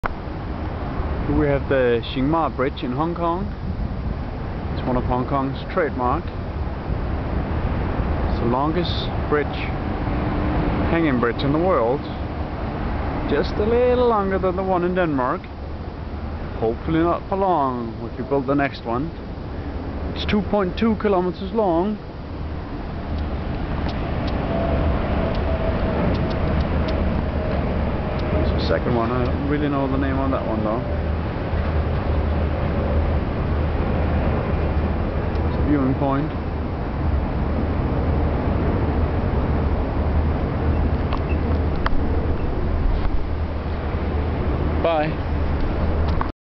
The longest hanging bridge in the world at night
My camera is not really fit for the dark, but this is the longest hanging bridge in the world, 2,2 kilometers. We could actually see three bridges from this viewpoint, nice ;-) enjoy
/9